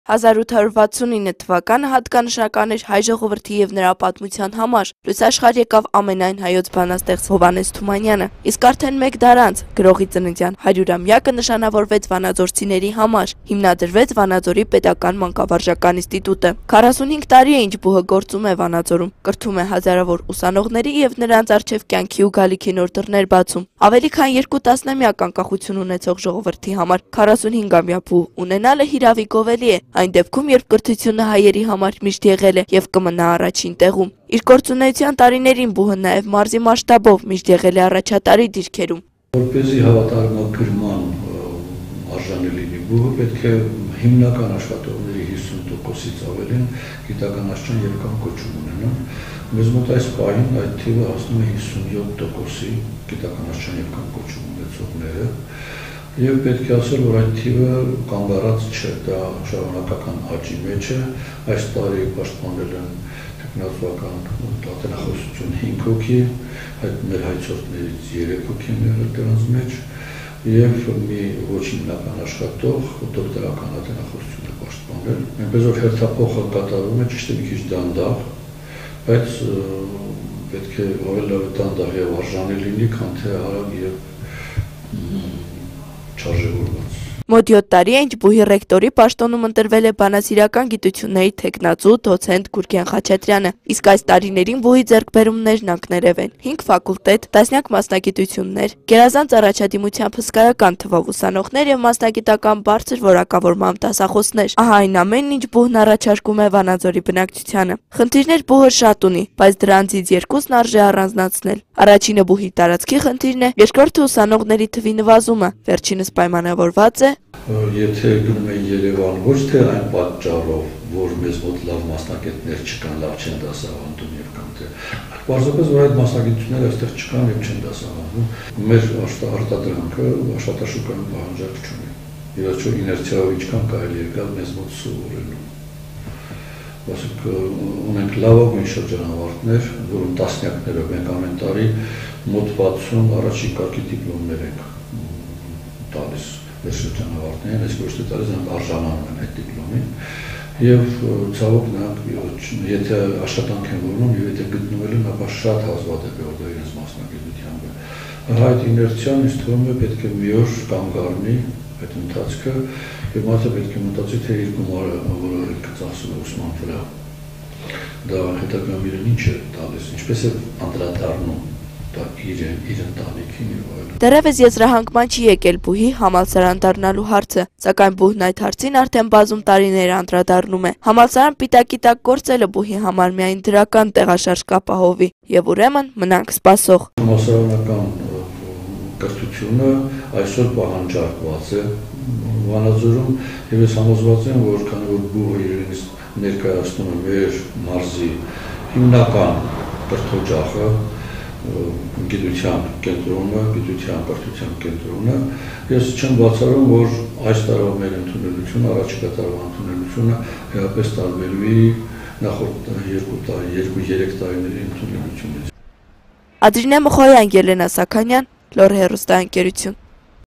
Վանաձորի Պետական Մանկավարժական Ինստիտուտը 45 տարվա պատմություն ունի
1869 թվականը հատկանշական էր հայ ժողովրդի և նրա պատմության համար. լույս աշխարհ եկավ «ամենայն հայոց բանաստեղծ» Հովհաննես Թումանյանը: Իսկ արդեն մեկ դար անց՝ գրողի ծննդյան 100 ամյակը նշանավորվեց վանաձորցիների համար. հիմնադրվեց ՎՊՄԻ-ին: 45 տարի է, ինչ բուհը  գործում է Վանաձորում, կրթում է 1000-ավոր ուսանողների և նրանց առջև կյանքի ու գալիքի նոր դռներ բացում: Ավելի քան 2 տասնամյակ անկախություն ունեցող ժողովրդի համար 45-ամյա բուհ ունենալը, հիրավի գովելի է, այն դեպքում, երբ կրթությունը հայերի համար միշտ եղել է և կմնա առաջին տեղում: Իր գործունեության տարիներին բուհը նաև մարզի մասշտաբով միշտ եղել է առաջատարի դիրքերում: